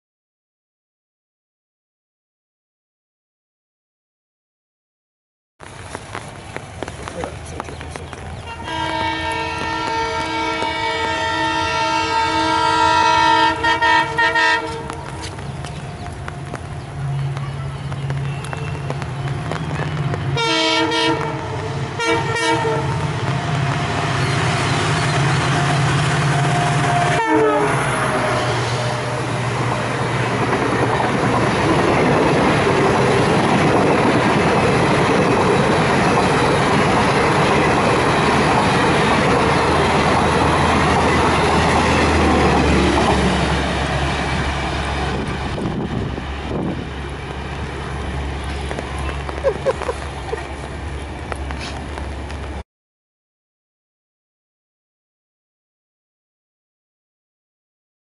An Angry EMD with Late running Garib Rath......
Super humming and honking effect by an EMD Monster. i.e PUNE WDG4 # 12502 as power of 1 hr late running 06511 Yesvantpur Jaipur (Weekly) Garib Rath Special angrily passed CDK and moving towards his next halt MSH.